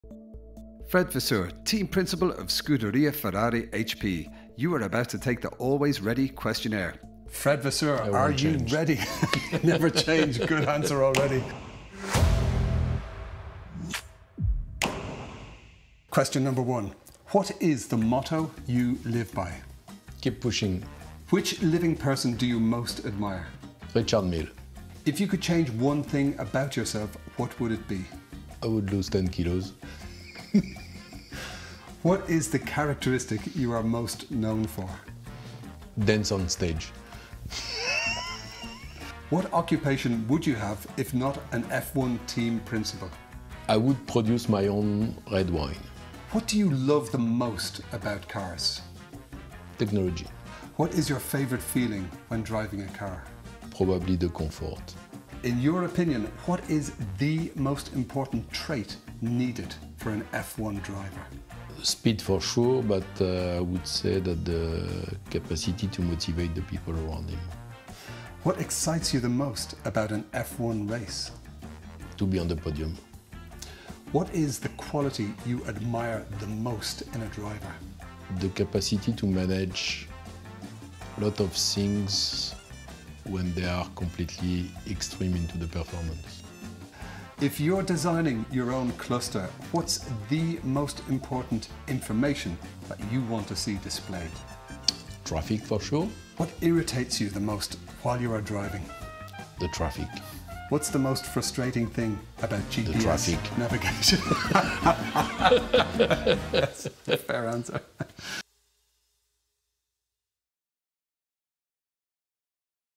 Always Ready Questionnaire - Frédéric Vasseur, Scuderia Ferrari HP, Interview
Frédéric Vasseur, Scuderia Ferrari HP Team Principal, answers some very interesting questions in the AlwaysReady Questionnaire.